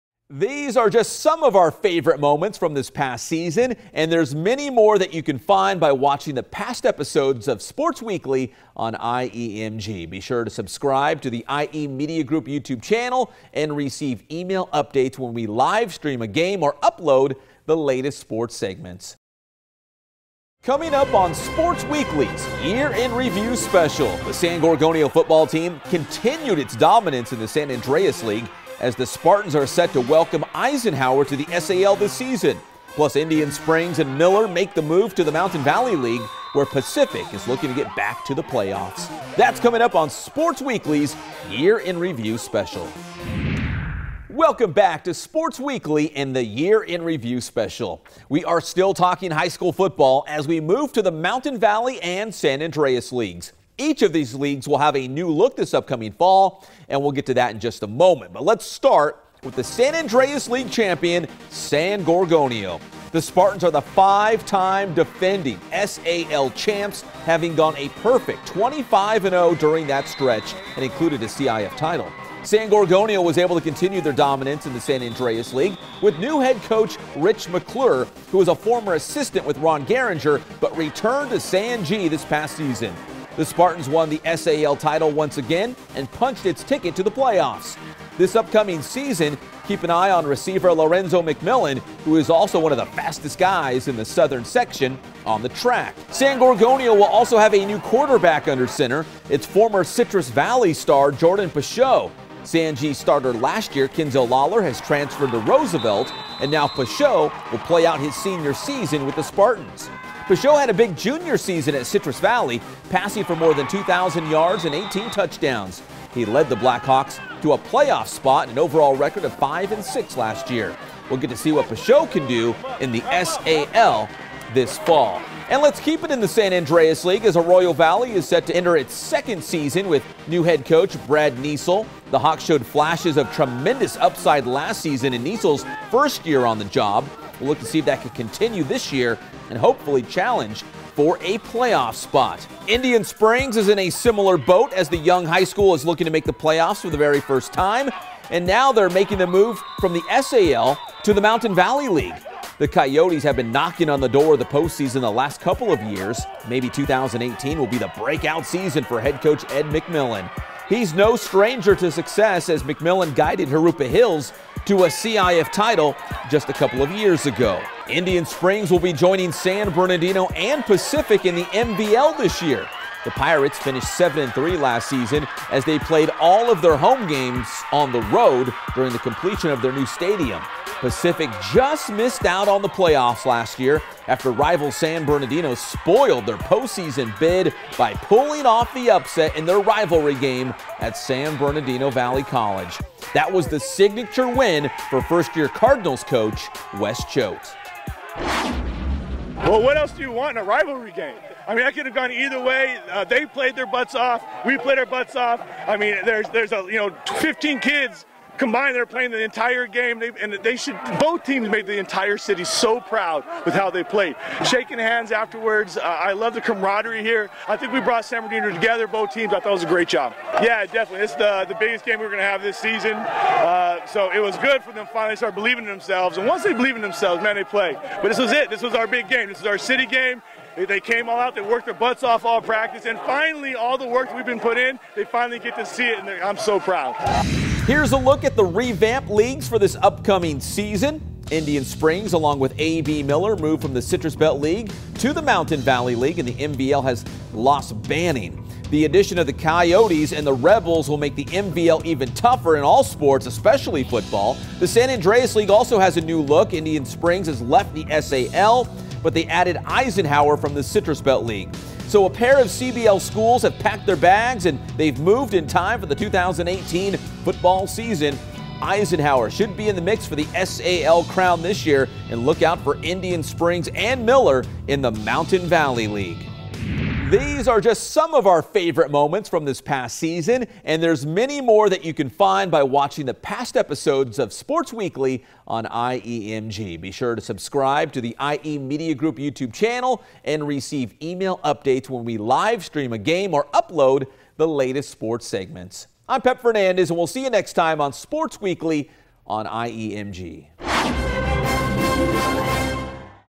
City of San Bernardino Sports Weekly FOOTBALL: New Look San Andreas & Mountain Valley Leagues
(San Bernardino, CA)
The San Gorgonio football team continued its dominance in the San Andreas League as the Spartans are set to welcome Eisenhower to the SAL this season. Plus, Indian Springs and Miller move to the Mountain Valley League where Pacific is looking to get back to the playoffs.